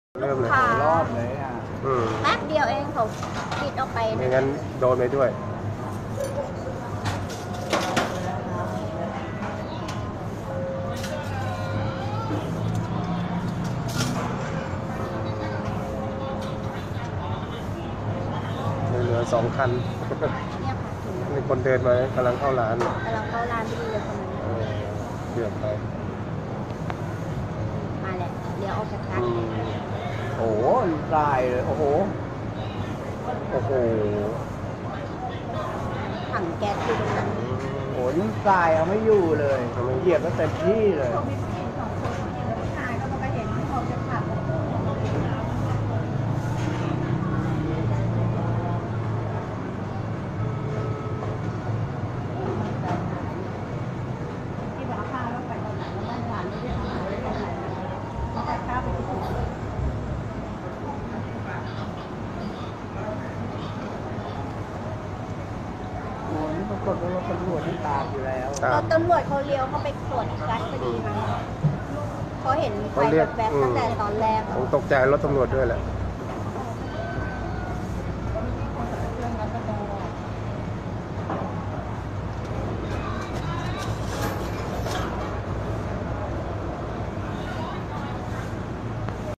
นาทีระทึก❗️ กระบะเสียหลัก_ชนเรียงคัน!!
เวลาประมาณ 01.30 น. นาทีระทึก ชนสนั่น !! หน้าร้านโกเทียร เมืองสุพรรณบุรี

ขอบคุณเรื่องแนะนำจาก–– @Thanut Nut Chiangmai